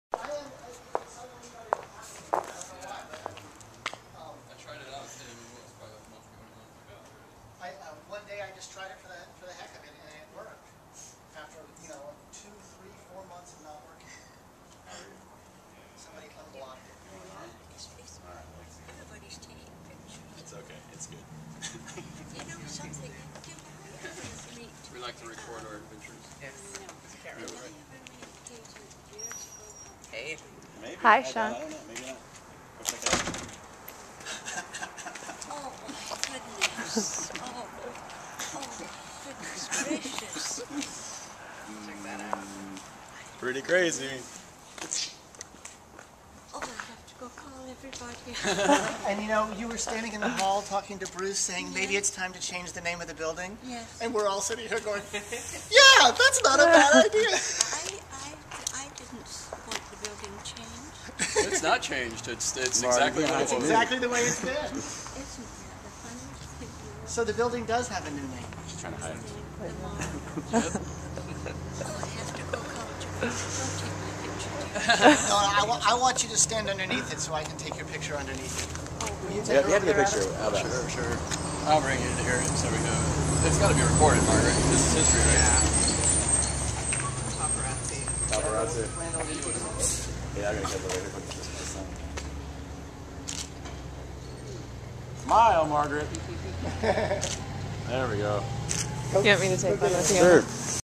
The Margaret
Renaming The Bel Baines apartment after 45+ year tenant, Margaret Mackie...